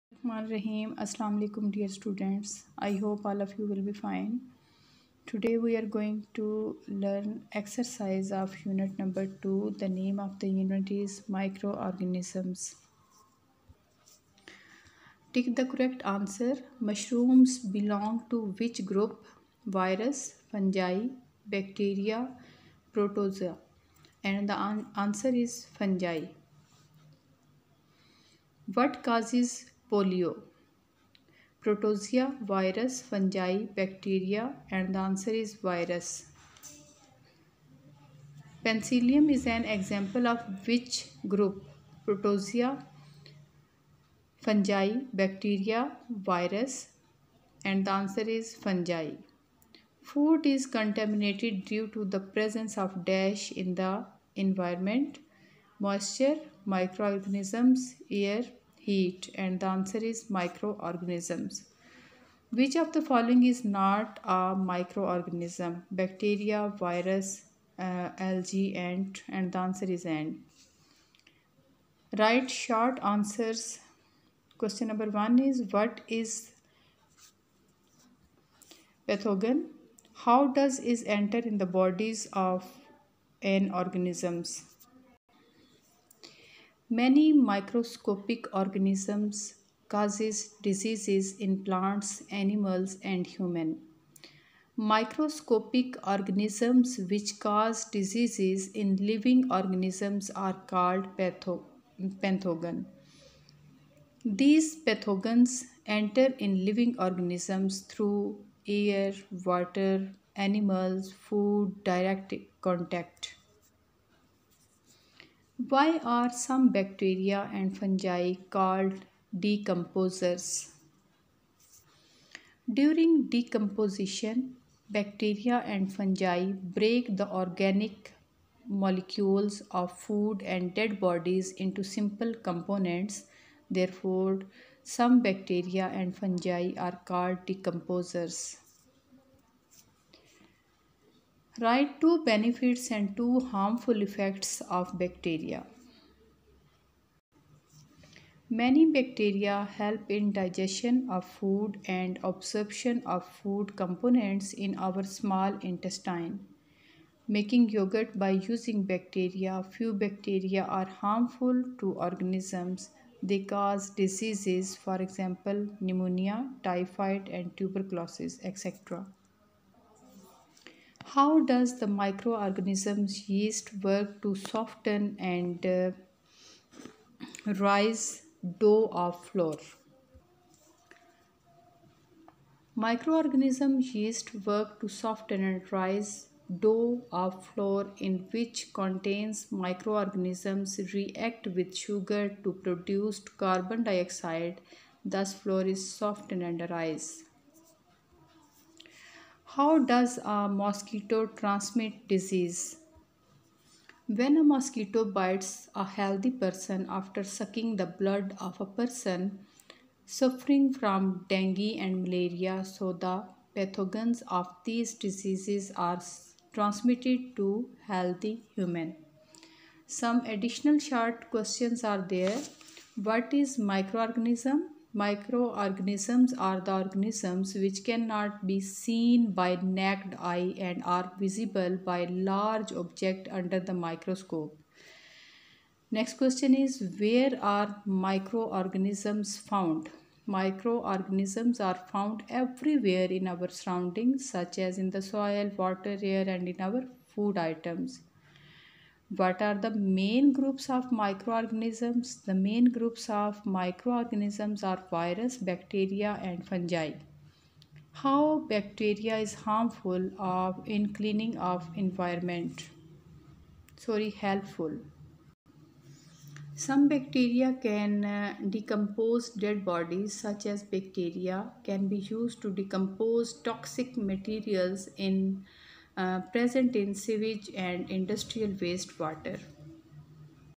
General Science Grade 5 Single National Curriculum Unit # 2 Microorganisms
1. General Science Grade 5
2. Based on Single National Curriculum 2020
3. Unit # 2 Microorganisms
4. Exercises
5. Tick the correct answer.
6. Write short answers.
7. Book page # 29, 30